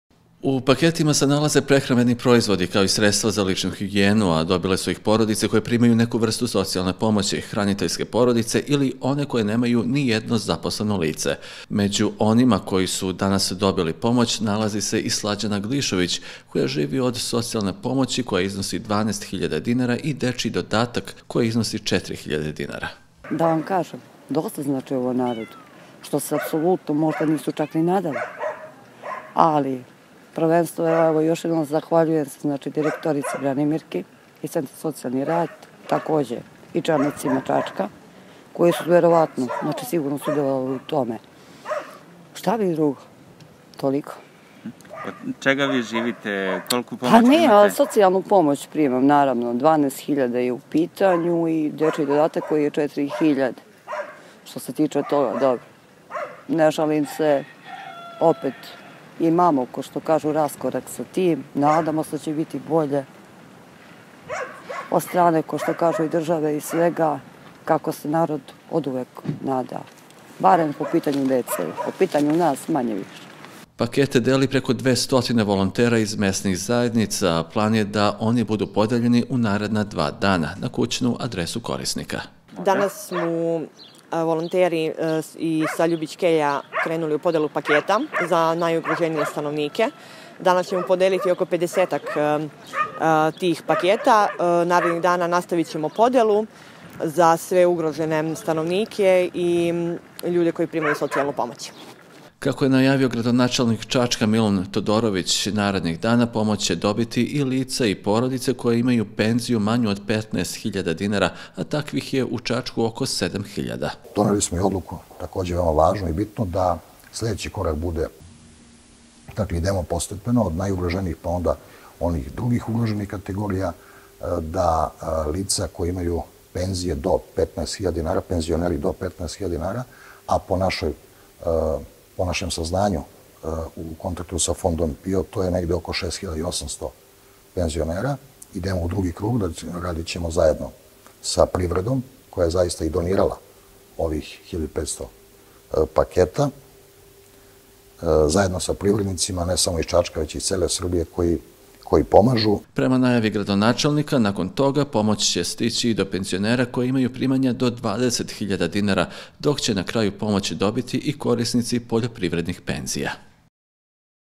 06.04.2020. U ČAČKU POČELA PODELA PAKETA POMOĆI ZA NAJUGROŽENIJE
U Čačku je danas počela distribucija 1.500 paketa pomoći za najugroženije građane, koje su obezbedili Grad Čačak i privrednici. Volonteri iz 11 centara tokom jutra su preuzeli pakete iz fiskulturne sale ustanove Zračak, a zatim krenuli sa podelom građanima koji se nalaze na spiskovima Centra za socijalni rad.

STUDIO ČAČAK

Adresa: Skadarska 7/3, 32000 Čačak

STUDIO UŽICE